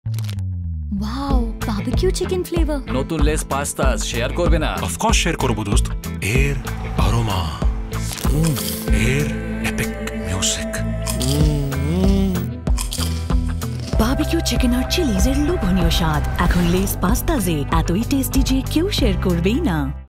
New Lay’s Pastazz American Style Barbecue Chicken
BBQ chicken ar chillies er lobhonio shaad. Ekhon Lay’s Pastazz-e.
Attoi tasty je, kau share korbei na